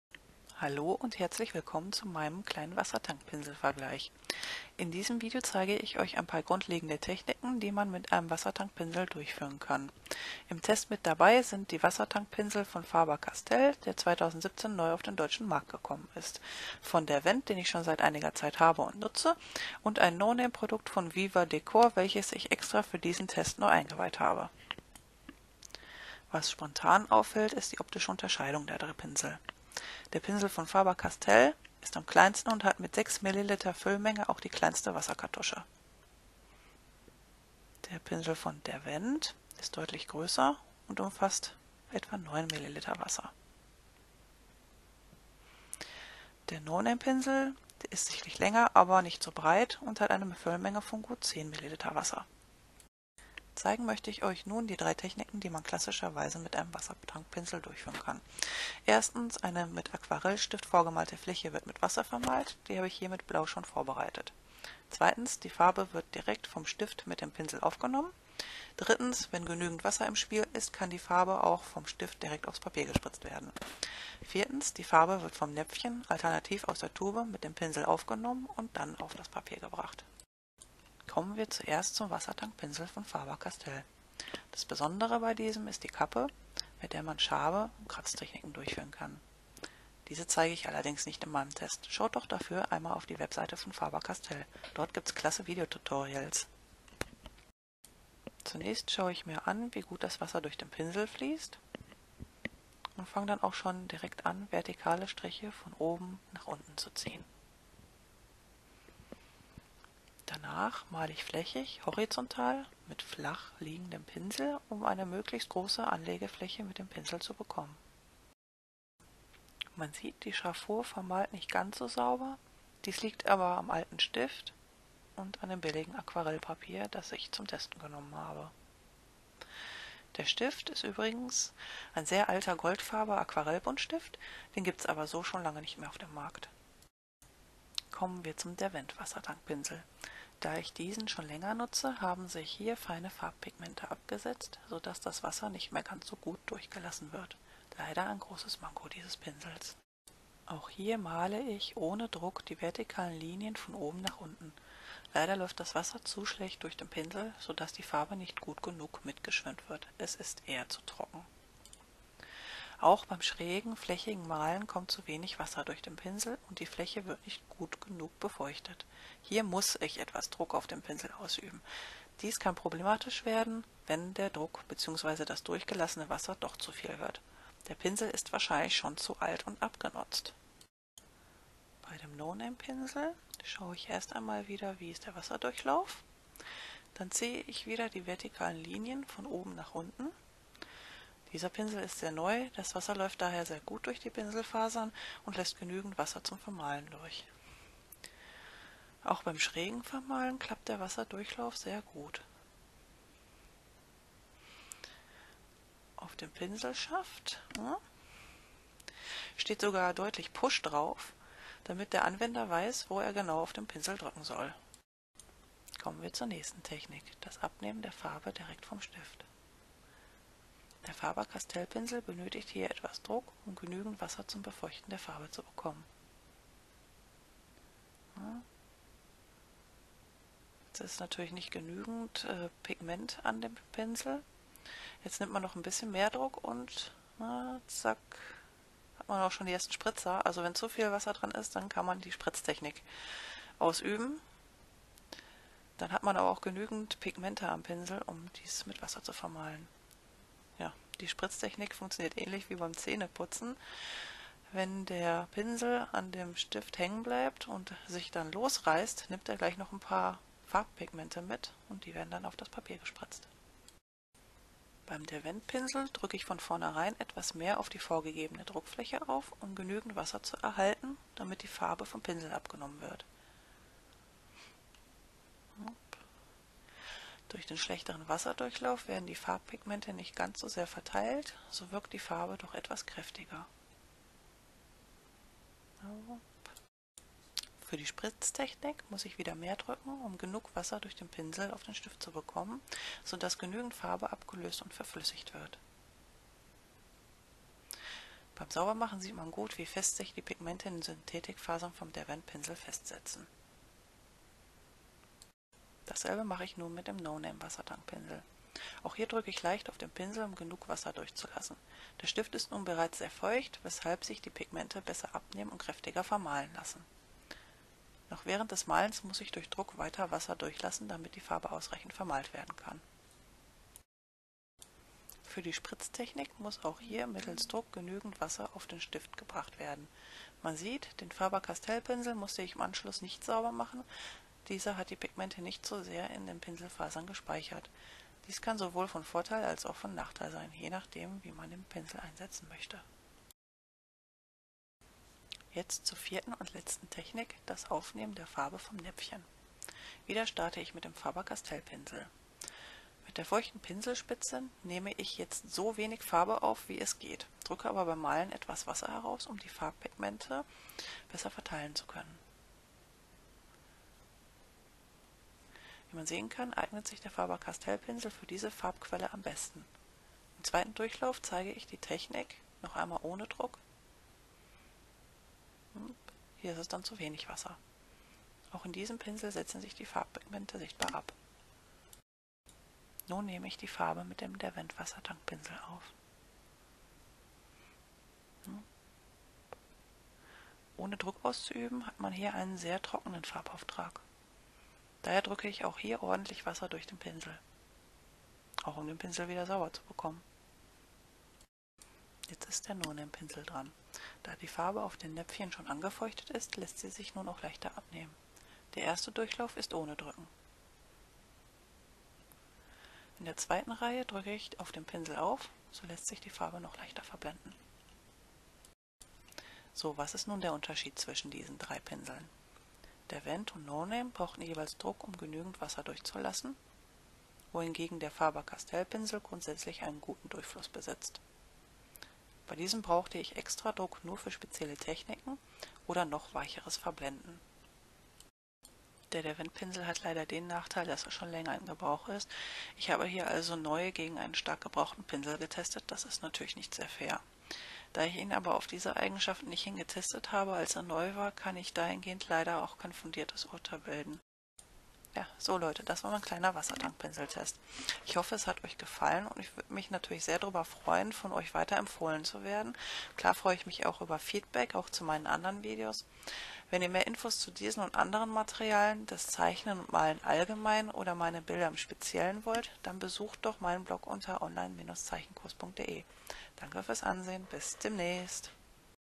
Der Faber-Castell Wassertankpinsel im Vergleich
In diesem Video zeige ich Euch ein paar grundlegende Techniken, die man mit einem Wassertankpinsel durchführen kann.

Im Test mit dabei sind – die Wassertankpinsel von Faber-Castell, der 2017 neu auf den deutschen Markt gekommen ist, von DERWENT, den ich schon seit einiger Zeit habe und nutze, und ein noName Produkt von Viva Decor, welches ich extra für diesen Test neu eingeweiht habe.

Zeigen möchte ich Euch die 3 Techniken, die man klassischerweise mit einem Wassertankpinsel durchführen kann:

1. eine mit Aquarellstift vor gemalte Fläche mit Wasser vermalen
2. die Farbe direkt vom Stift mit dem Pinsel aufnehmen
3. oder sogar die Farbe vom Stift aufs Papier spritzen
4. die Farbe vom Näpfchen, alternativ aus der Tube, mit dem Pinsel aufnehmen

Mehr Informationen zum Wassertankpinsel von Faber-Castell findet Ihr hier in meinem Blog